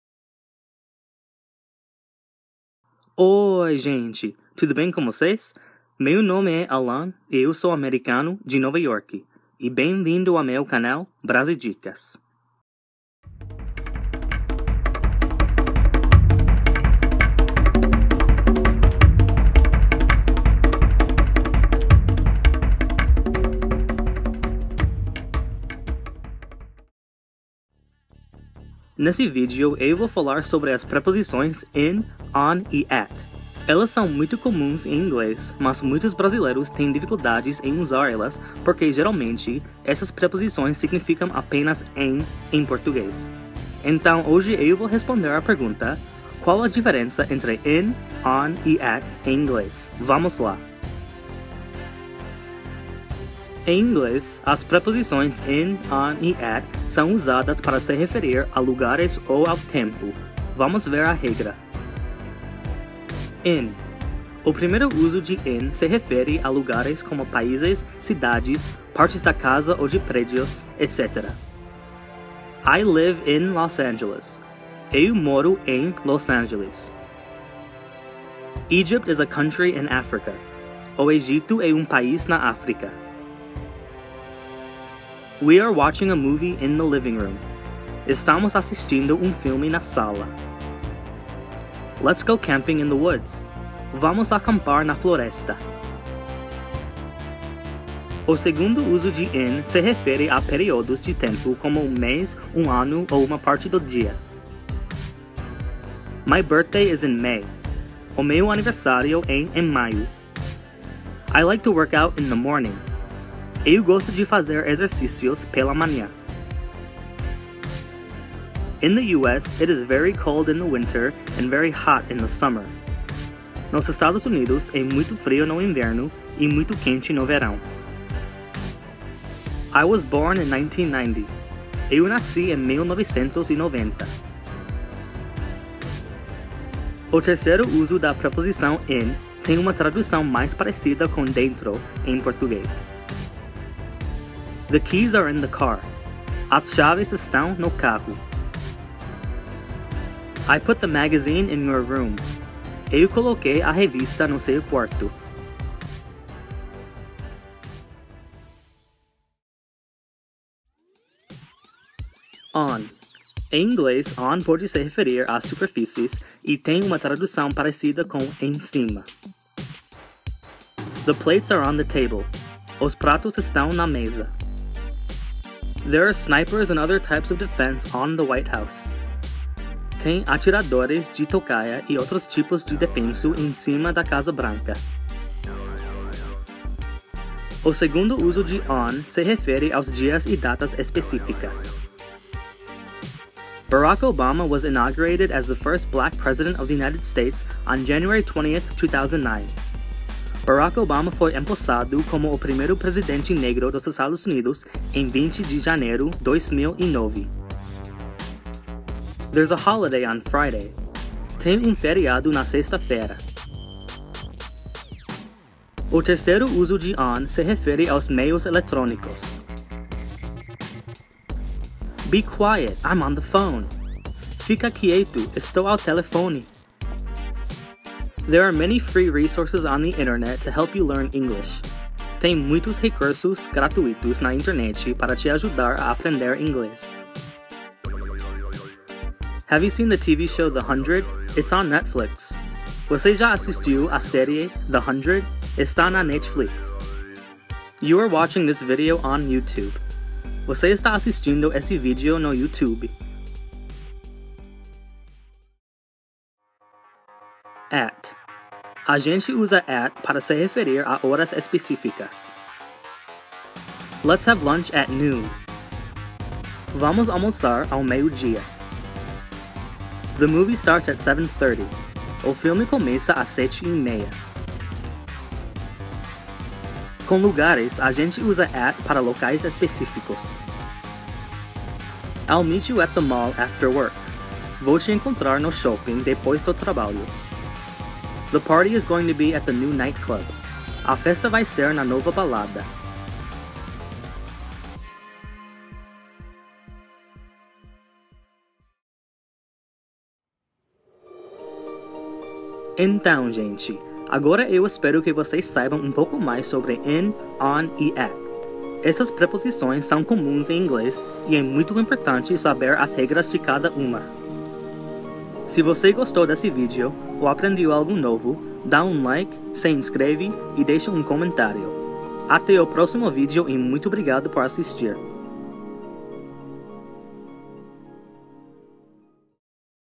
Qual A Diferença Entre 'In', 'On', E 'At' Em Inglês?
Nesse vídeo eu explico a diferença entre 3 preposições em inglês que significam 'em' em português.

Música-